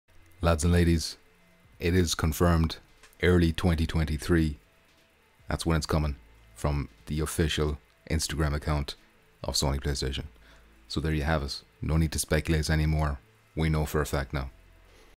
PSVR 2 Is Coming Early 2023
No need to speculate anymore as Sony reveal the launch window for PSVR2.